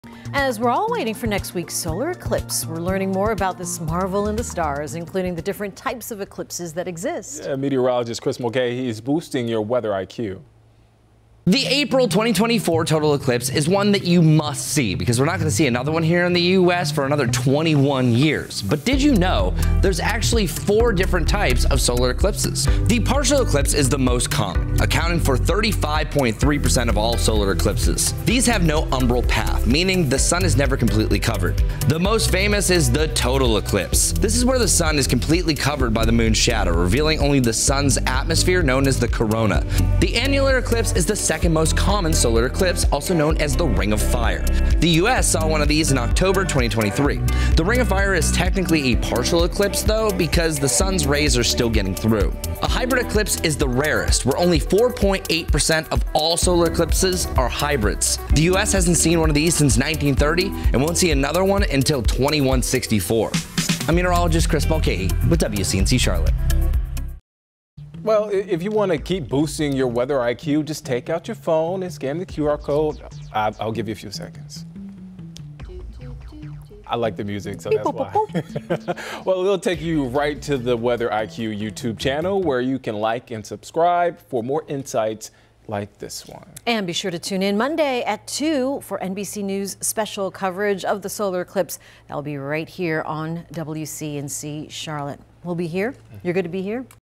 2024 Solar Eclipse | What are the different kinds of solar eclipses?
The total solar eclipse is less than a week away.

The last time the United States experienced a total solar eclipse was six and a half years ago – Aug. 21, 2017.

The next one isn't until 2044, so this is a rare event.

But did you know there are four different types of solar eclipses?